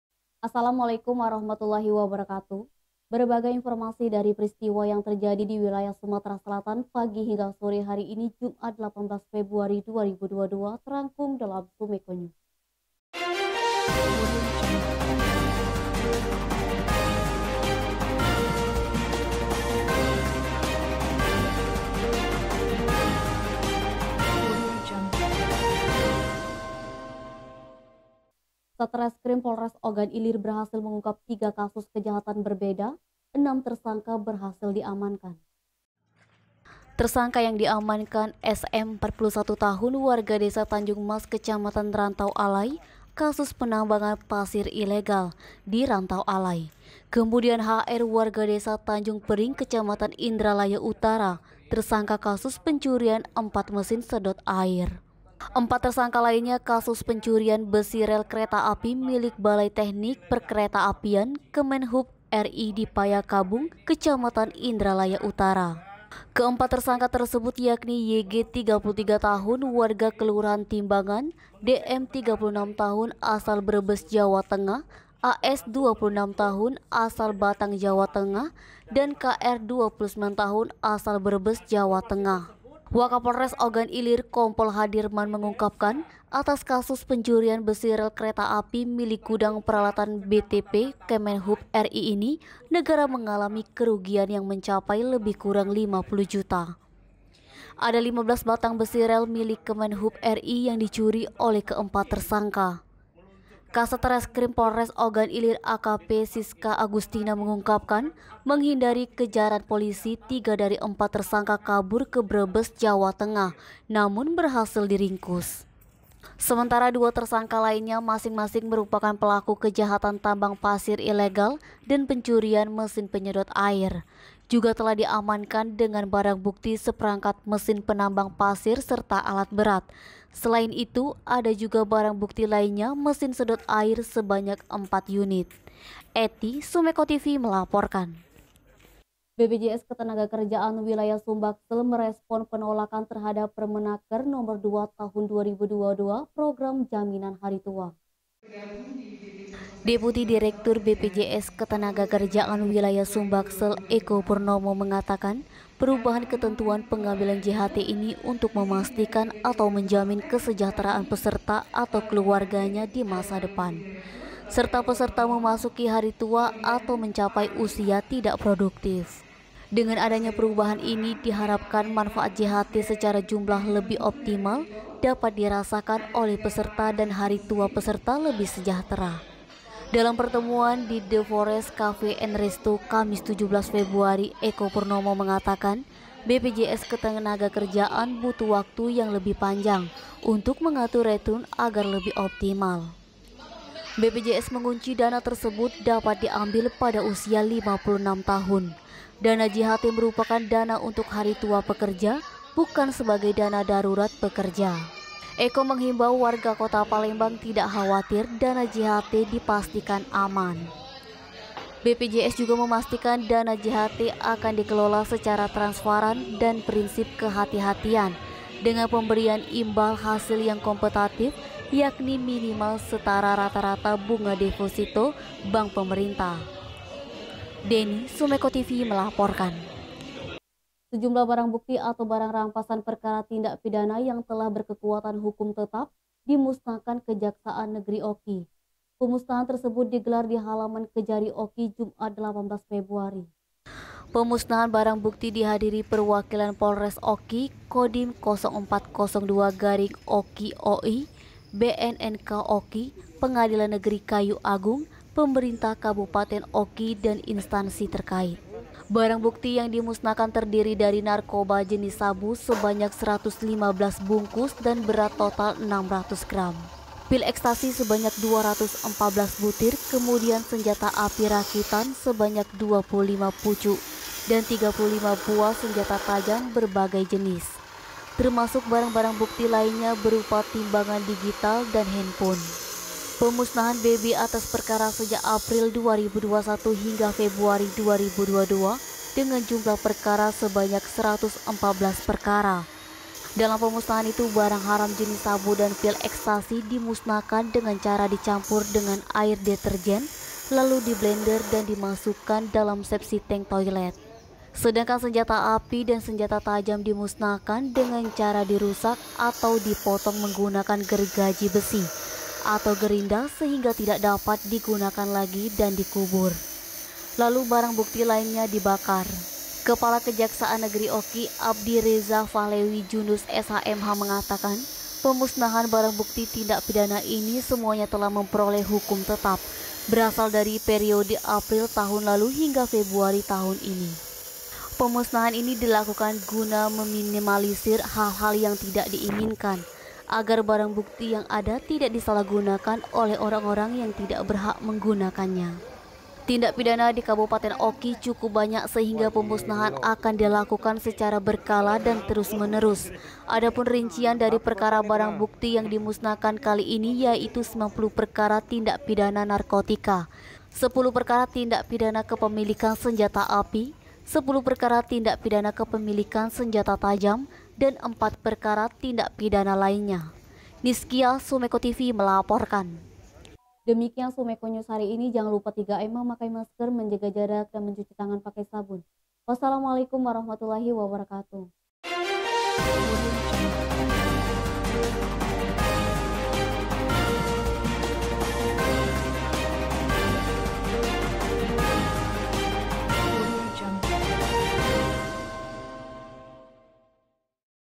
POLRES OGAN ILIR TANGKAP 6 PELAKU dari 3 KASUS KEJAHATAN
Satreskrim Polres Ogan Ilir berhasil menangkap enam tersangka dengan tiga kasus kejahatan yang berbeda sekaligus.

Hal itu terungkap dari press release yang digelar pada Kamis (17/2) petang di Mapolres Ogan Ilir,.